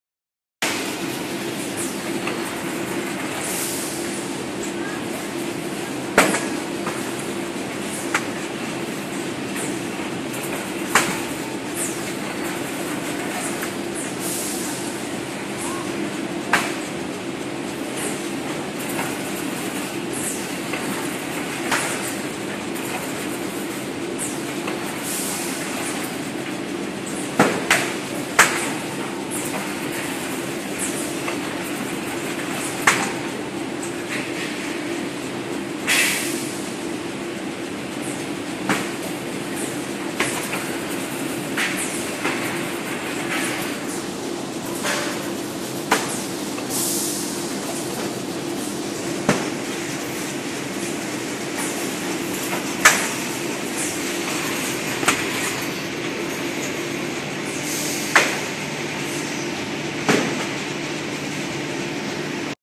cardboard auto bundling machine;corrugated box strapping machine;cardboard tying machine
auto bundling machine; auto strapping machine, auto tying machine,auto packaging machine;
Fully auto tying machine inline flexo printer,it is professional to supply solutions for you.
1 Adopted double servo, double press, double push, double open structure, applied for corrugated cardboard and boxes
2 Strapped single and double tie packaging
3 Feeding unit and front rack adopted mounting bracket, back rack adopted double open single press, which will be increased packaging speed and steady,
4 Low cost: The cost of PE belt was 3-5 times lower than the PP belt
5 Good stability: Imported servo control system helps to reduce the mechanical drive and Mechanical wear, expand the lifespan
6 Humanization design: automatic counting and production yield
7 Multifunction HMI, easy to operate, imported programmable fault monitoring system
8 Left and right head can be adjusted independently, easily and versatile operating
9 Finishing tie is more feasible, when knotting it will be far away from machine for preventing box damaged.
10 Used Schneider electric brand( Servo, switches, proximity switch, limit switch, contactors); all packaging sizes are set by computer; SMC pneumatic flutter made products look neat artistic
11 Conveyor belt are imported from Italy